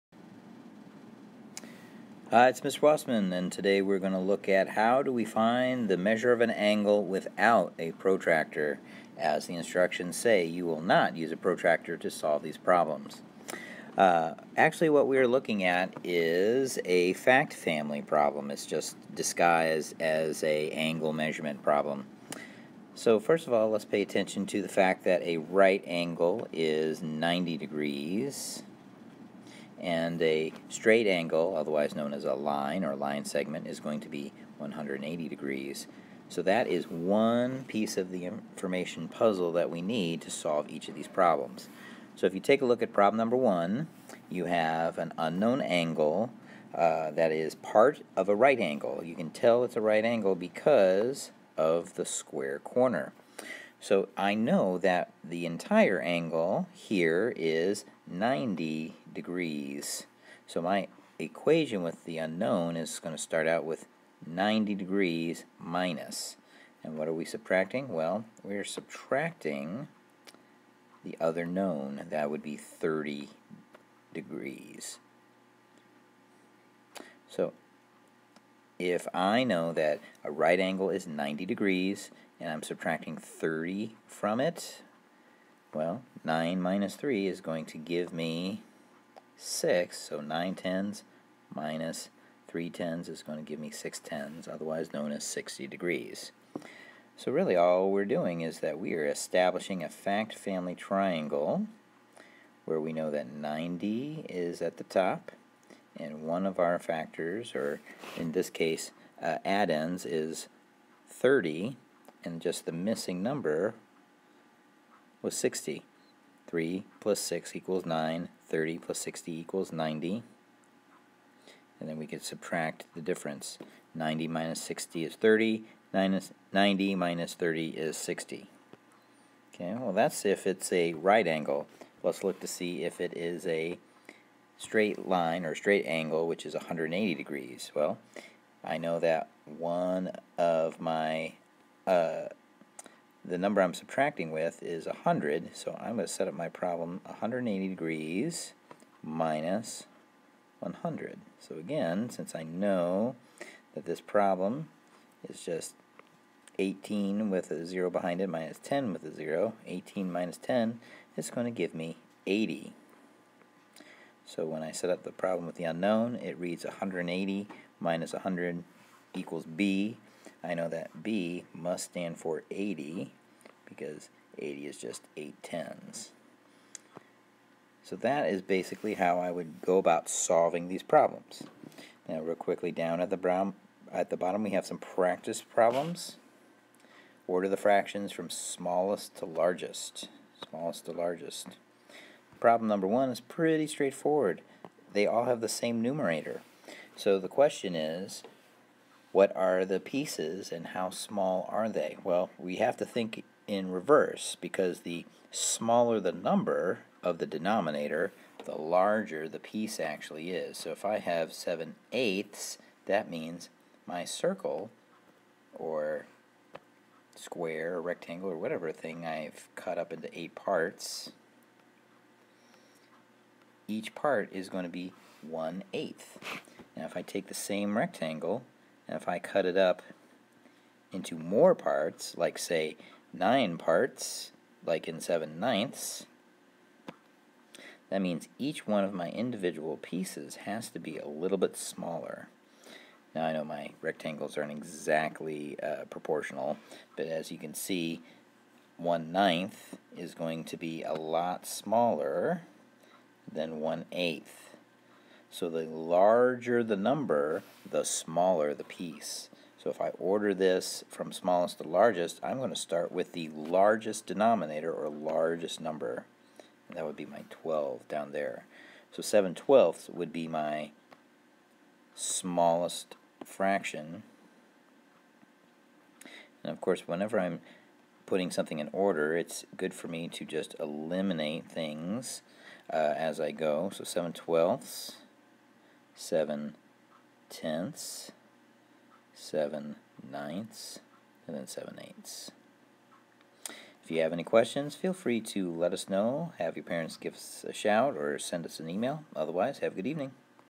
Everyday Math, 4th Grade Home Links 6.11 "Finding Angle Measures" (without a Protractor) page 181
Mr. Wasemann shows us how to use the concept of fact families to determine the missing measure of an angle within a right angle or straight angle.